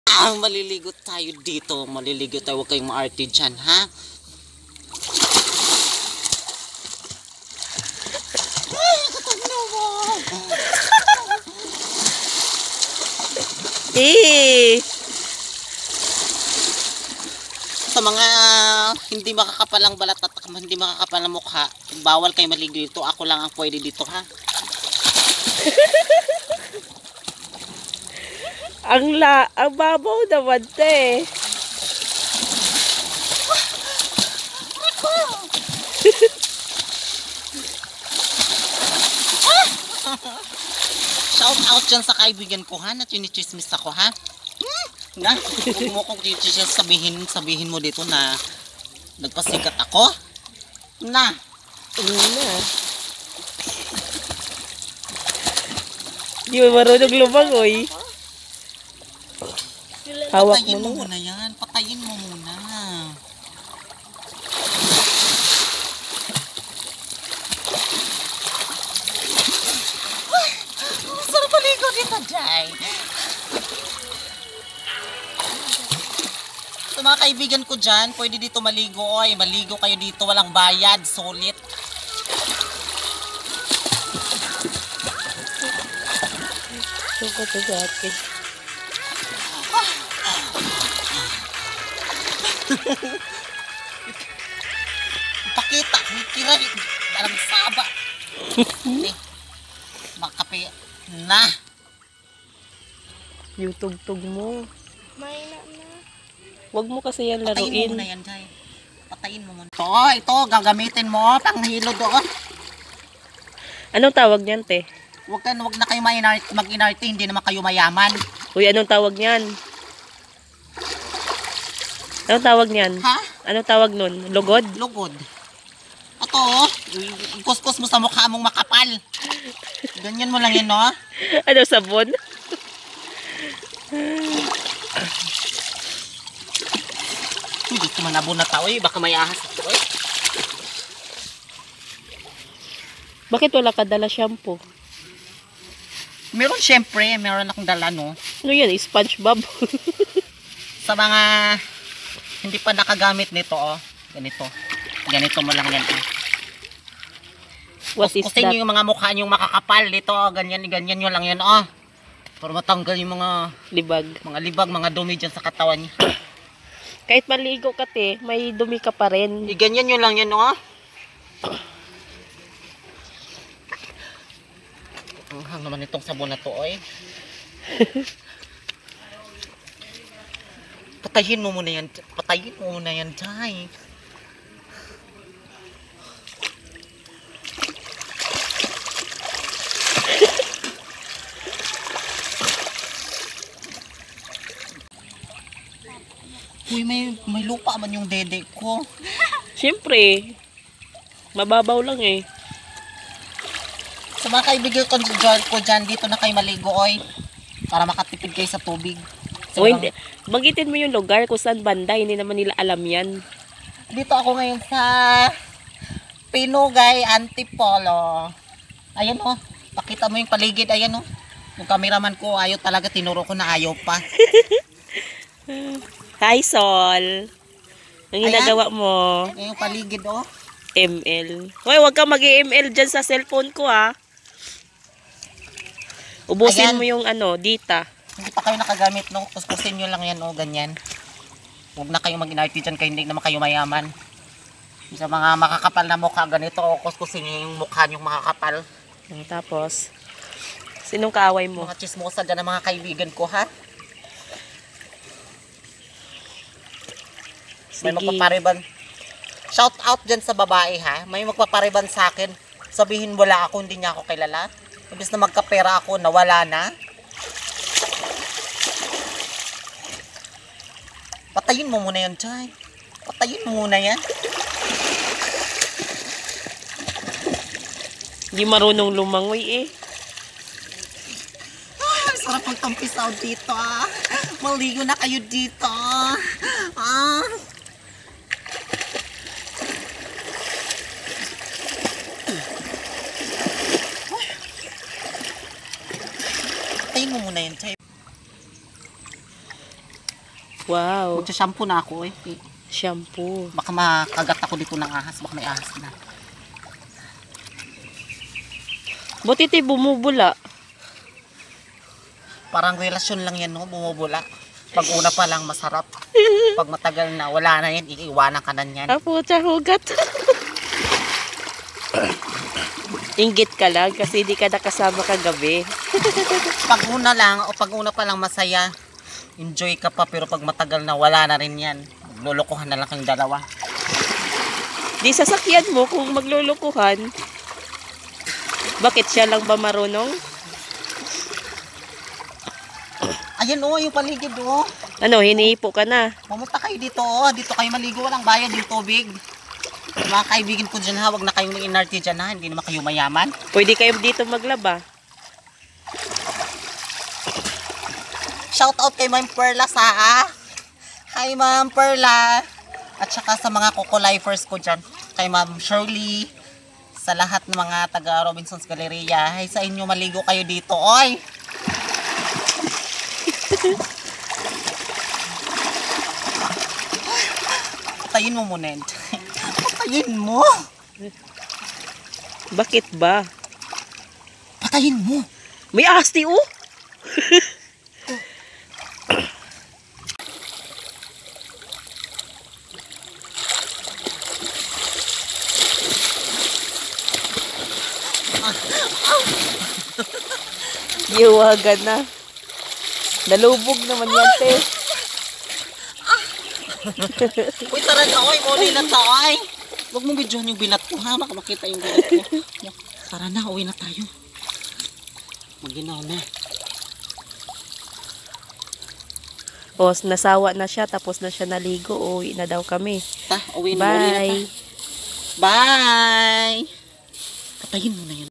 DITO NA KAYO MALIGO SULIT WALANG BAYAD | Pinugay ANTIPOLO RIZAL
maligo tayo dito oi kasi walang bayad makatipid pa kayo sa tubig